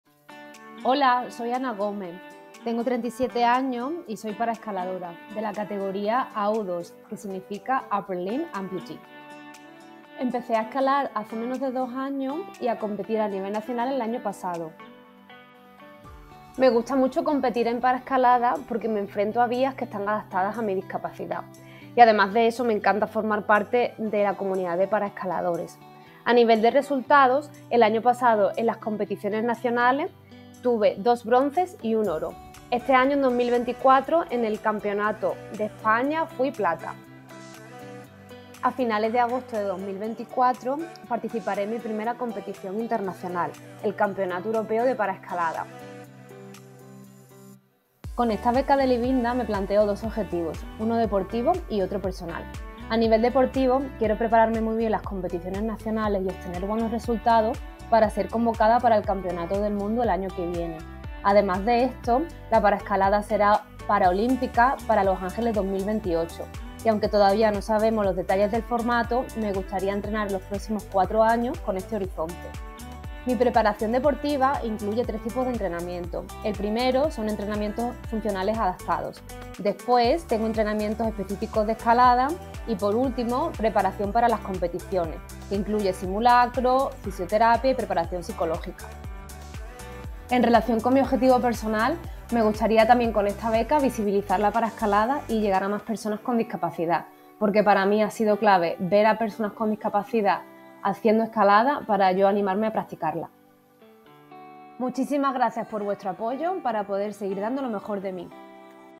TanAnaClimb, Ana Gómez Escobar (37)
Ana Gómez practica paraescalada desde 2022. Quiere visibilizar la paraescalada y poder atraer a más personas con discapacidad para que se animen a escalar. Uno de sus objetivos, ser convocada para el Campeonato del Mundo de Paraescalada en 2025. Para lograrlo, necesita de nuestro apoyo. ¿Quieres conocer su historia?